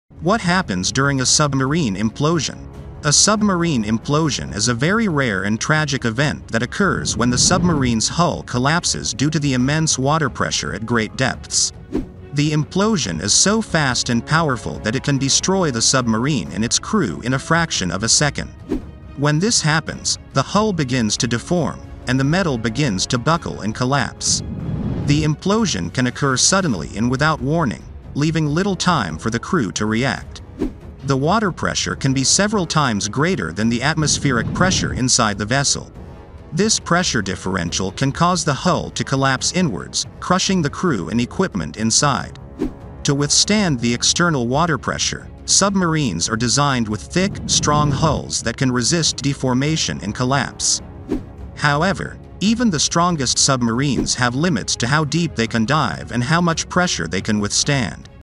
What Happens During a Submarine Implosion?
A submarine implosion is a very rare and tragic event that occurs when the submarine's hull collapses due to the immense water pressure at great depths. The implosion is so fast and powerful that it can destroy the submarine and its crew in a fraction of a second.
When this happens, the hull begins to deform, and the metal begins to buckle and collapse. The implosion can occur suddenly and without warning, leaving little time for the crew to react.
The water pressure can be several times greater than the atmospheric pressure inside the vessel. This pressure differential can cause the hull to collapse inwards, crushing the crew and equipment inside.
To withstand the external water pressure, submarines are designed with thick, strong hulls that can resist deformation and collapse.
However, even the strongest submarines have limits to how deep they can dive and how much pressure they can withstand.

IMPORTANT !!